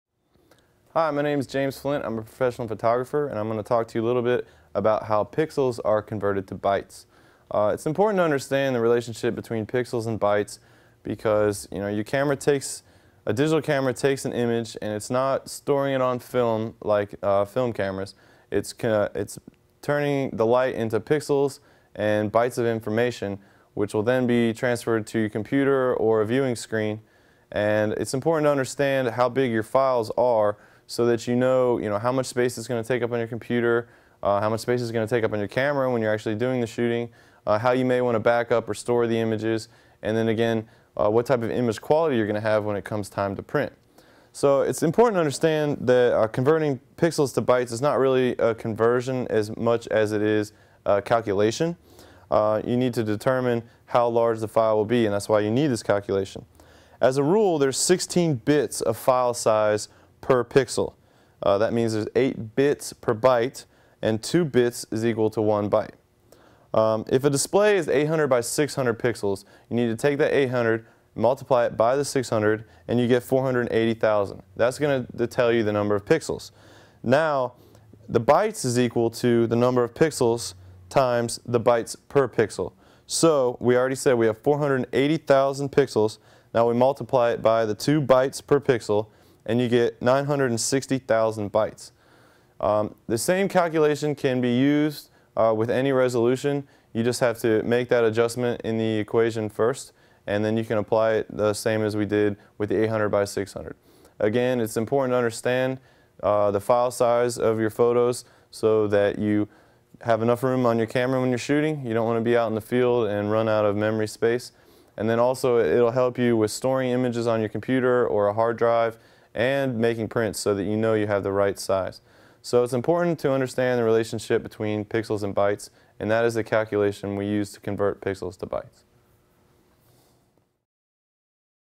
Digital Camera Technology : How Pixels Are Converted to Bytes
Converting pixels to bytes requires calculating the number of pixels multiplied by the bytes per pixel. Use the standard information that there are two bytes per pixel to complete the equation. Calculate how many bytes a photo will be with information from a professional photographer in this free video on photography.